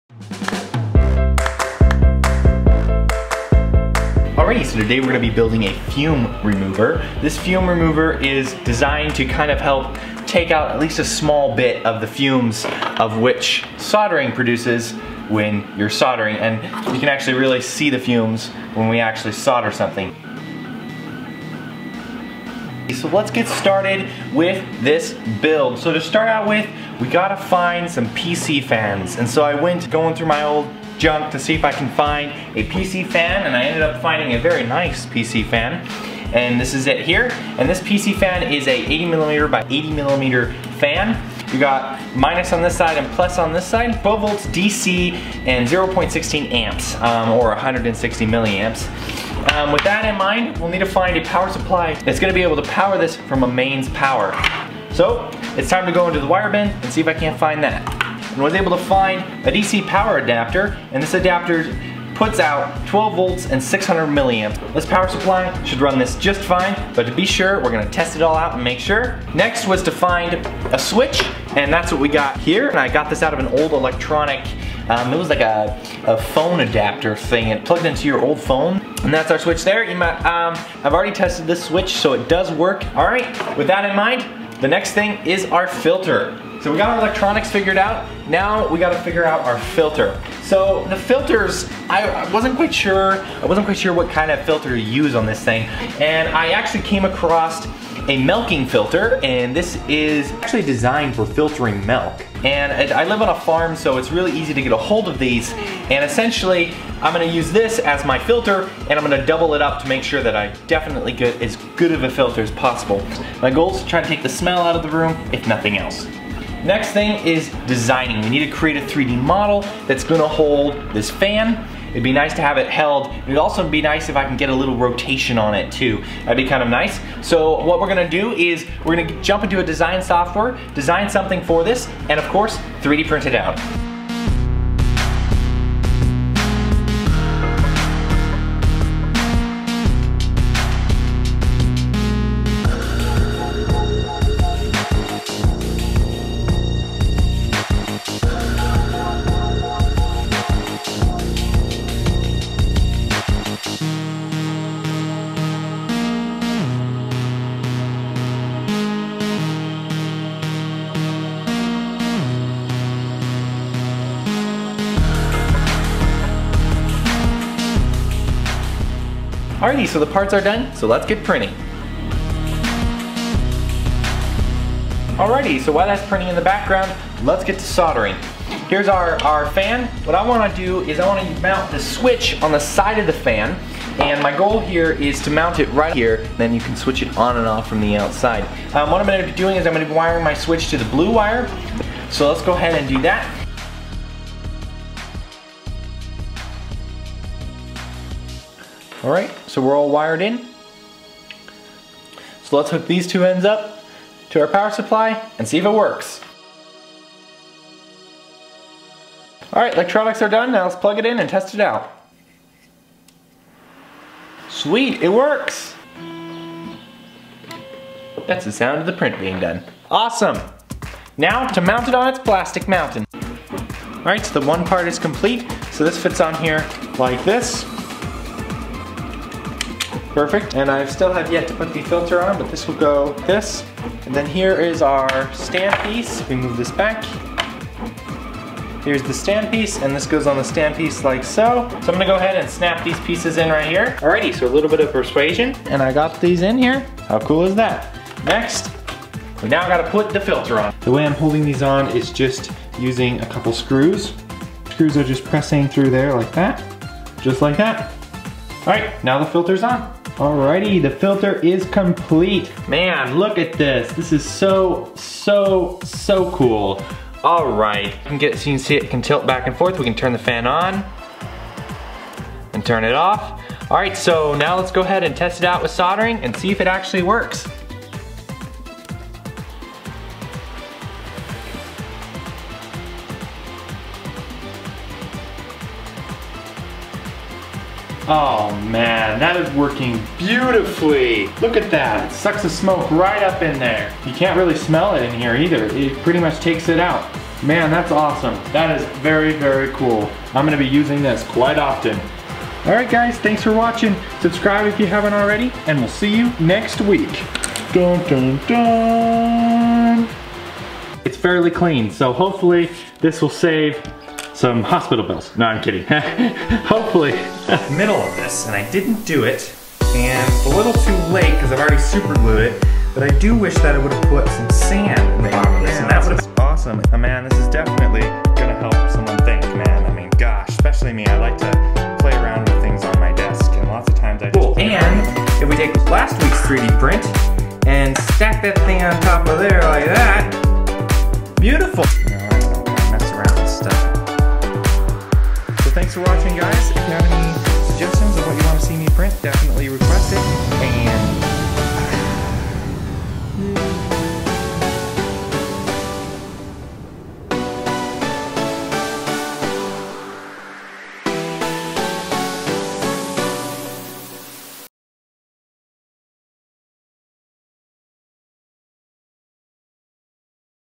3D Printed Soldering Fume Remover!
I designed and 3d printed this because I wanted to get rid of most of the fumes in my room.